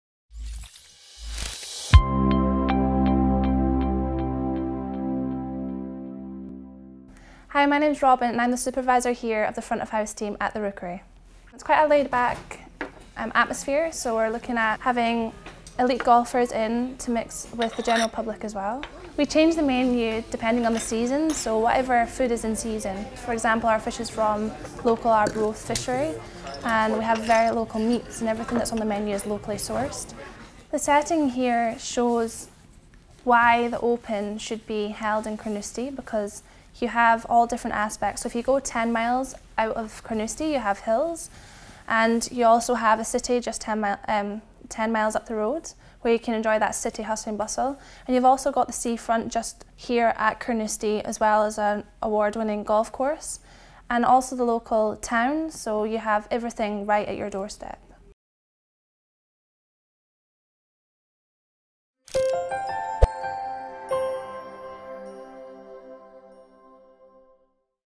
CCDT 6 ROBYN PINDER at the ROOKERY
Carnoustie Shorts

Robyn Pinder - the 6th in CCDT's Carnoustie Shorts series introducing local people doing extraordinary things - here we meet Robyn Supervisor, Front of House, Rookery Restaurant.

Produced by Carnoustie Community Development Trust in collaboration with Dylan Drummond of Son of the Sea the interviews paint a portrait of each person as if you've been invited into their home to get to know them over a cup of tea – informal, conversational, warm, authentic, natural.

CCDT wish to thanks the sponsors whose donations have made this project possible - Angus Council and Carnoustie Golf Links.